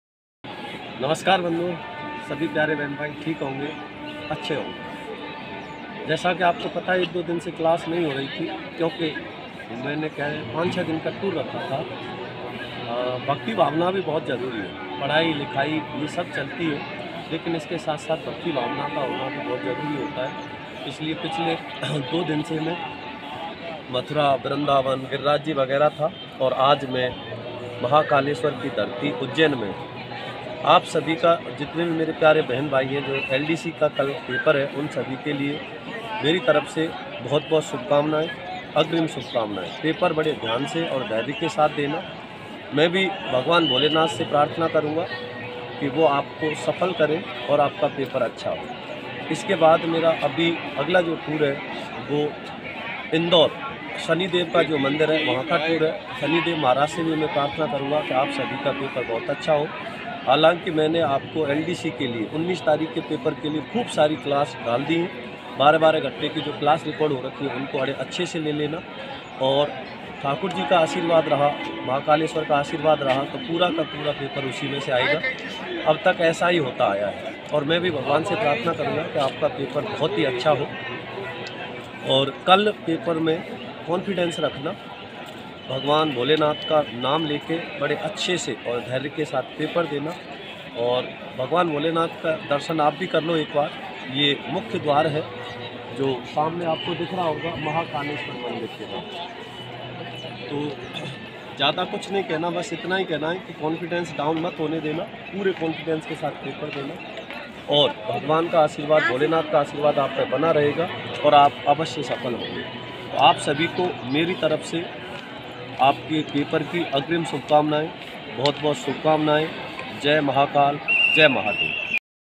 विद्यार्थी हित के लिए सहदेव सर ने की 'भगवान महाकाल' से प्रार्थना || SAHADEV SIR
हाई कोर्ट की कल होने वाली परीक्षा तथा आगामी समस्त परीक्षाओं में शामिल होने वाले विद्यार्थियों के उज्जवल भविष्य के लिए तथा परीक्षा दे चुके विद्यार्थियों के लिए उनकी उचित सफलता हेतु भगवान महाकाल से प्राथना की।

आप सभी को आपकी मेहनत का उचित फल मिले तथा आपका तथा आपके माता पिता का देखा गया सपना साकार हो।

ऐप को डाउनलोड करने के लिए इस लिंक पर क्लिक करें।